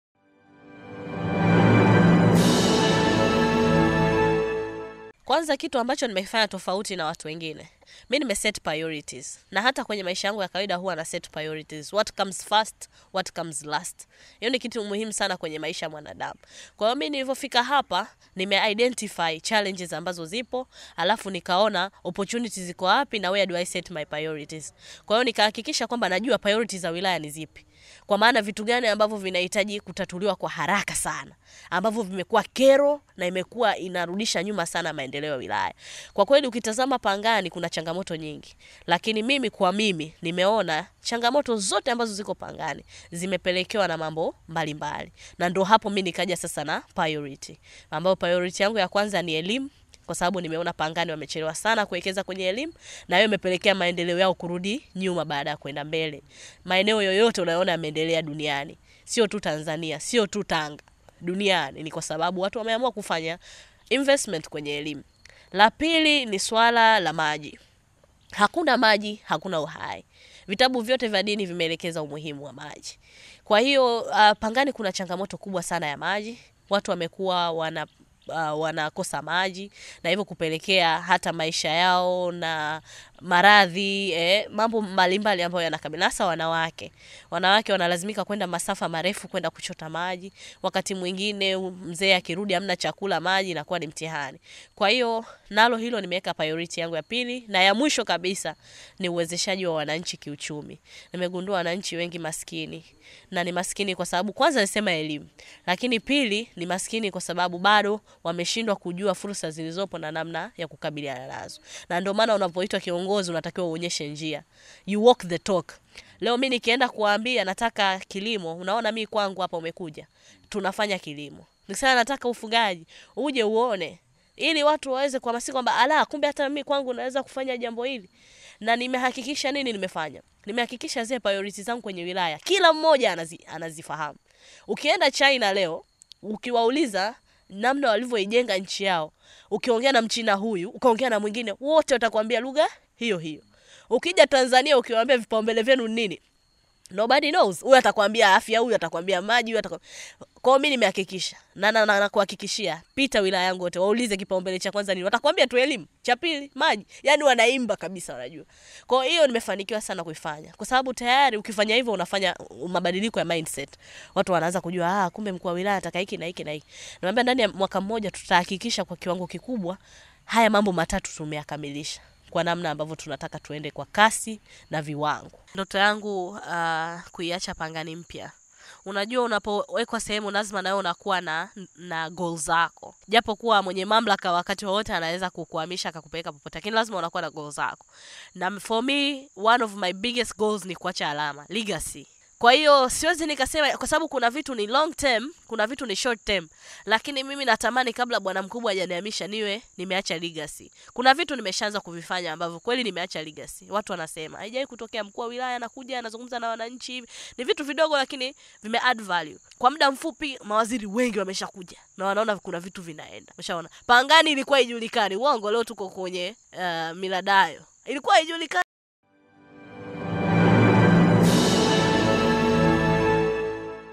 DC mdogo kuliko wote TZ kazungumzia anachojivunia kwenye wilaya yake
Mkuu wa wilaya ya Pangani mwenye umri mdogo zaidi, Zainab Abdallah amekaa na AyoTV kwenye EXCLUSIVE interview na kueleza anachojivunia kwenye wilaya yake ndani ya mwaka mmoja.